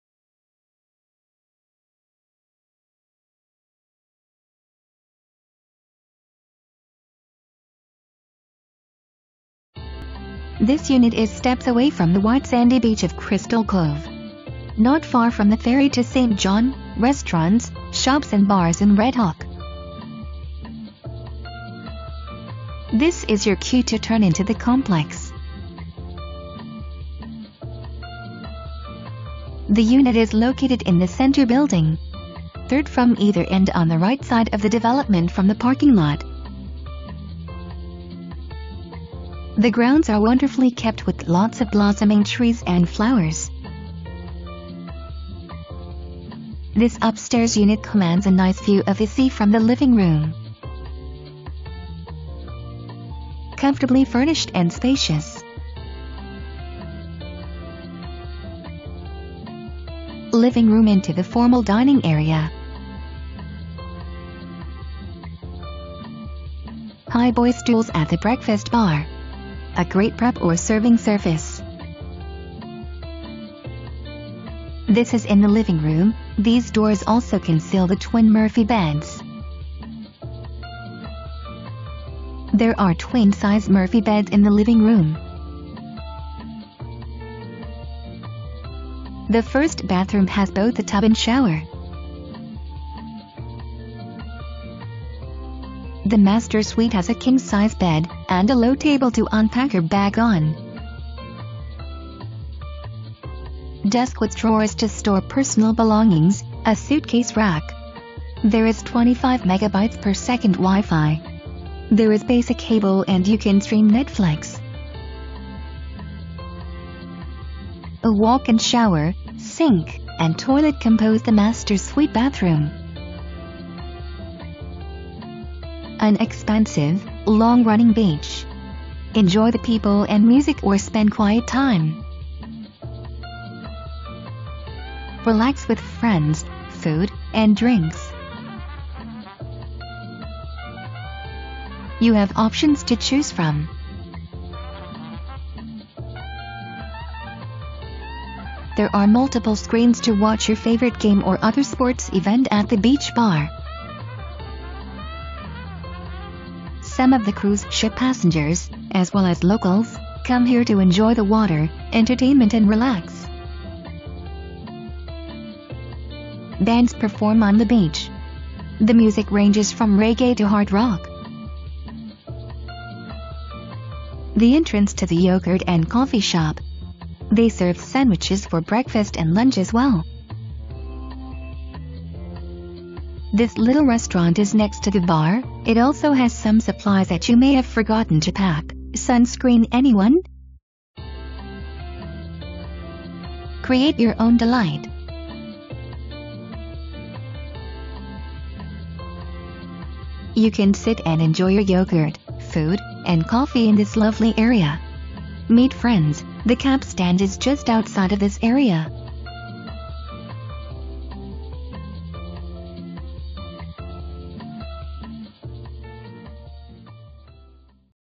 1 Bedroom 2 bath Condo Steps from the Water
Ciao Bella
St. Thomas, VI 00802
1 bed, 1.75 bath

On Crystal Cover a well-appointed condo, fully furnished, sleeps 5 with murphy beds and pull out couch. Steps from the white sandy beach and warm waters. WIFI. Close to the ferry to St. John and the BVI's, restaurants, and bars in Red Hook.

Tanya Van Blake-Coleman
Van Blake-Coleman Realty